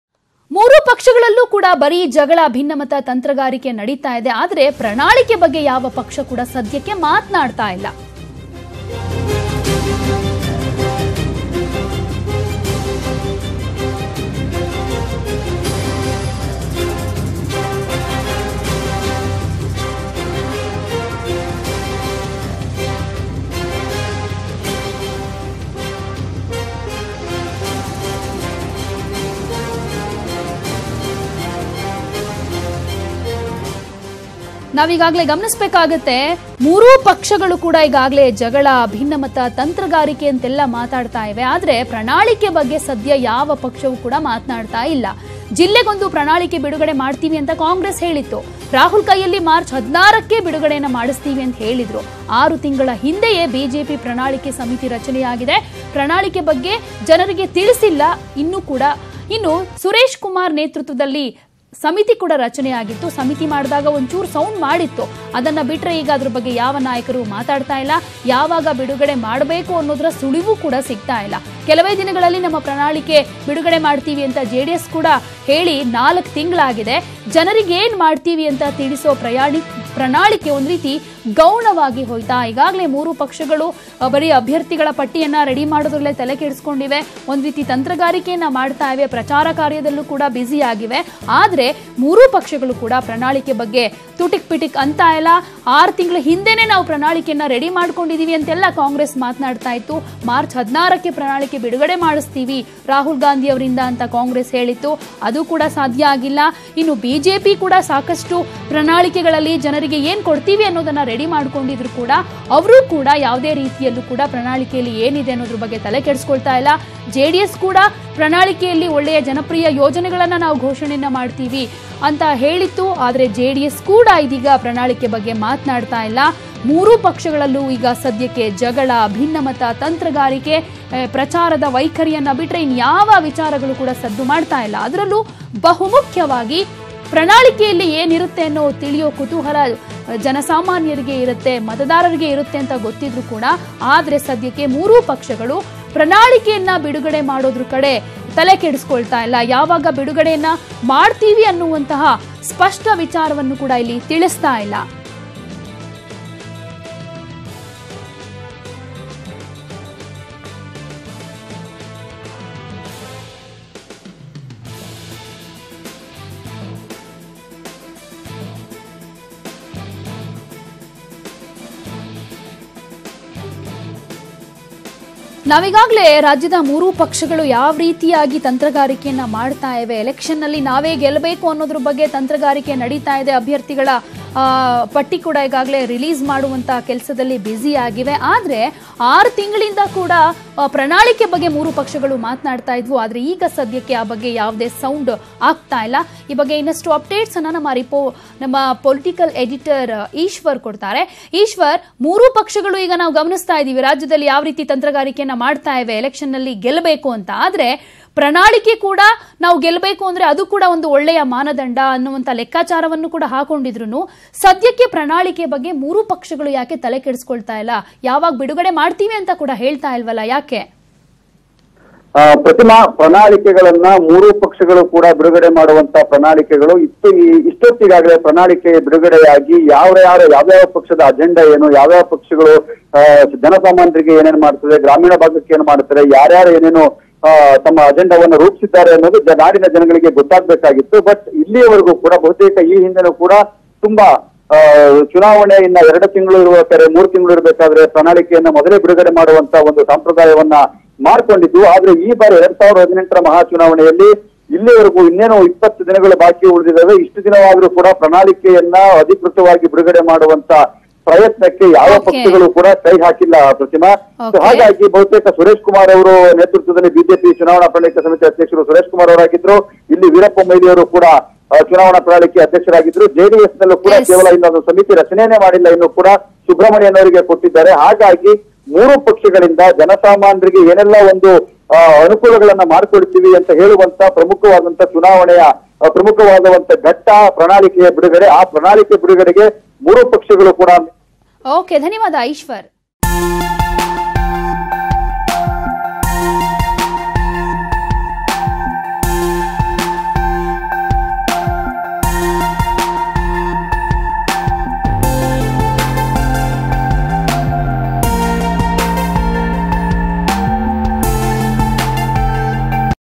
Karnataka Elections: When Will The Manifestos Be Released..?
When will the Congress, BJP & JDS parties release the manifestos needed for the people..?